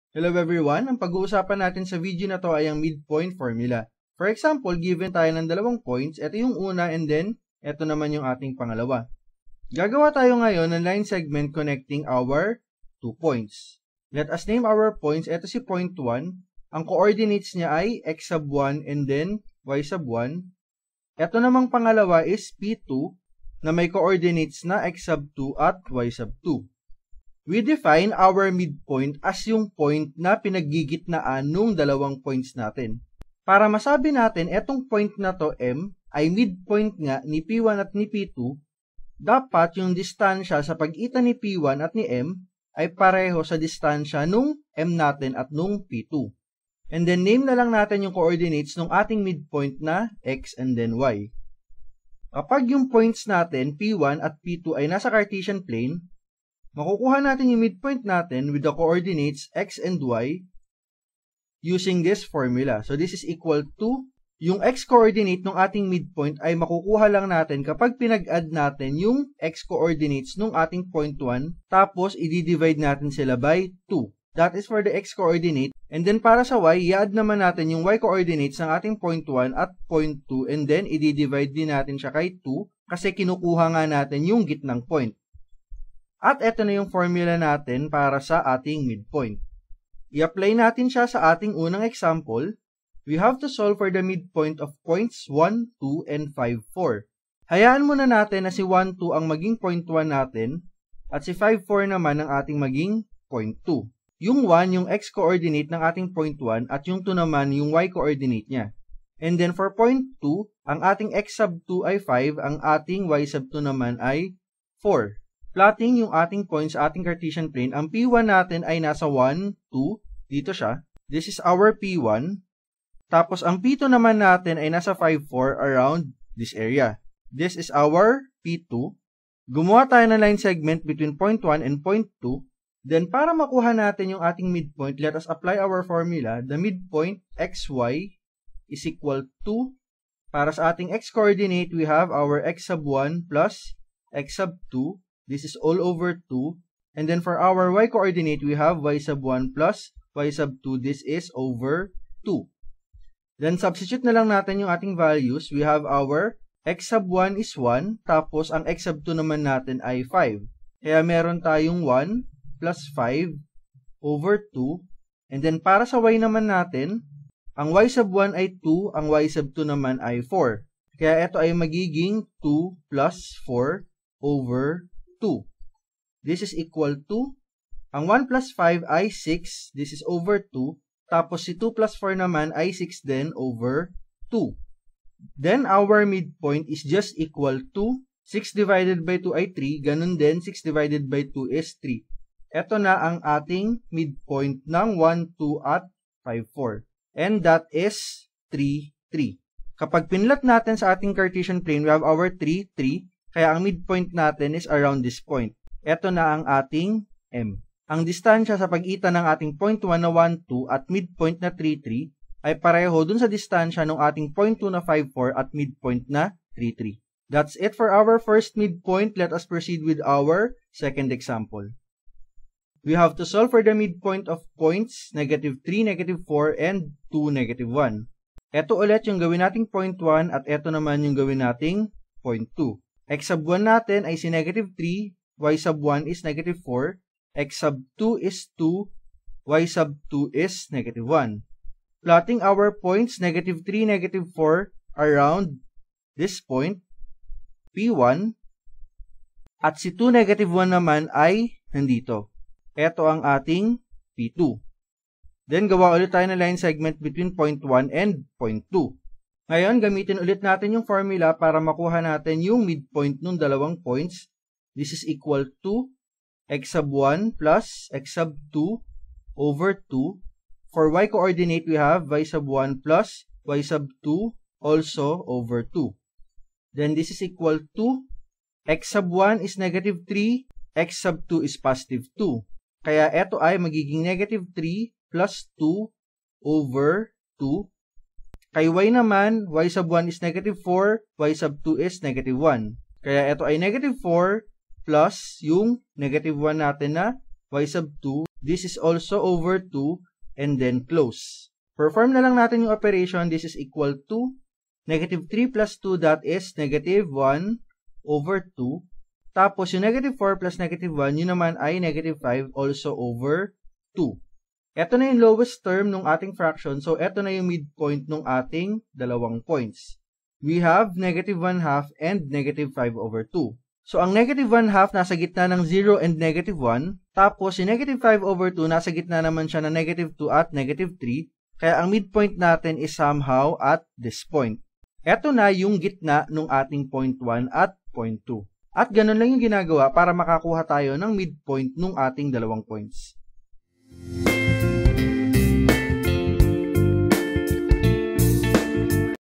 Midpoint Formula - Pre-Calculus / Analytic Geometry
A video solving for the distance between two points of horizontal, vertical and diagonal line segments (using Distance Formula). Designed for Flipped Classroom.